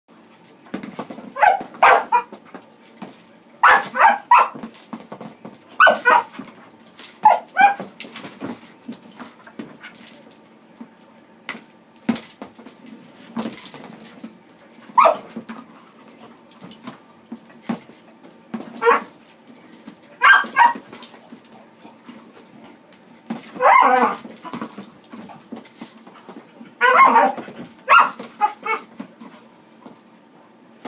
Wobbler toy, Cattle Dog in a tub!
Cattle Dog pup loves his Kong Wobbler toy, but looses it under things- so we  put it in a rubber tub for him!  What fun he has getting the treats out of his Wobbler now!  :)